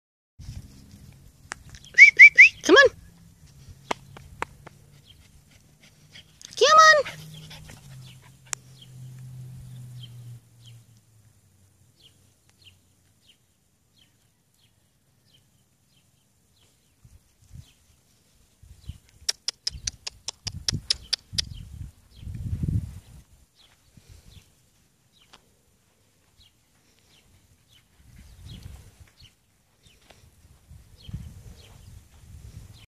Rat Terrier puppies
Playing in the back yard